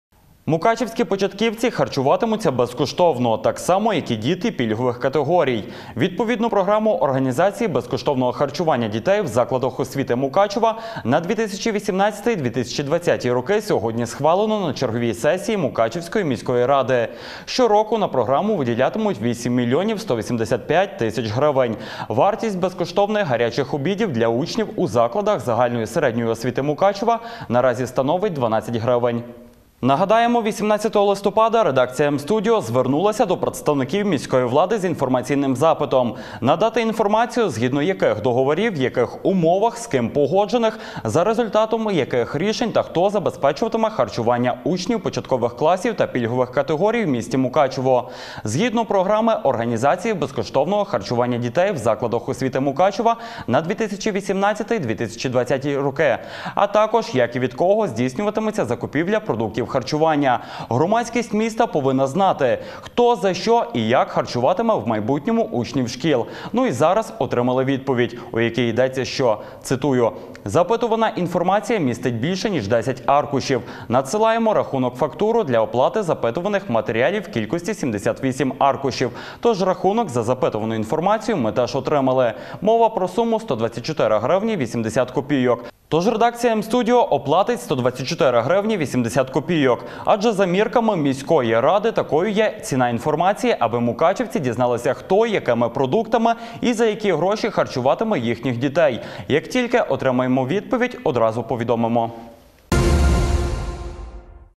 Харчування школярів в Мукачеві
Мукачівські початківці харчуватимуться безкоштовно, так само, як і діти пільгових категорій. Відповідну програму організації безкоштовного харчування дітей у закладах освіти Мукачева на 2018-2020 роки сьогодні схвалено на черговій сесії міськради. Щороку на програму виділятимуть 8 млн. 185 тис.гривень. Вартість безкоштовних гарячих обідів для учнів у закладах загальної середньої освіти Мукачева наразі становить 12 гривень. Нагадаємо, 18 листопада редакція М-студіо звернулась до представників міської влади з інформаційним запитом, надати інформацію, Згідно яких Договорів, у яких умовах, з ким погоджених, за результатами яких рішень та хто забезпечуватиме харчуванням учнів початкових класів та пільгових категорій у м. Мукачево, згідно програми організації безкоштовного харчування дітей у закладах освіти Мукачева на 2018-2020 роки, а також як і від кого здійснюватиметься закупівля продуктів харчування. Громадськість міста повинна знати, хто, за що і як харчуватиме у майбутньому учнів шкіл. Ну і зараз отримали відповідь, у якій йдеться, що , цитую: «запитувана інформація містить більше, ніж 10 аркушів, надсилаємо рахунок-фактуру для оплати запитуваних матеріалів в кількості 78 аркущів» Тож, рахунок за запитувану інформацію ми теж отримали, мова про суму 124 грн 80 копійок. Тож редакція М-студіо оплатить 124 грн 80 копійок, адже, за мірками міської ради такою є ціна інформації, аби мукачівці дізналися хто , якими продуктами і за які гроші харчуватиме їхніх дітей. Як тільки отримаємо відповідь – одразу повідомимо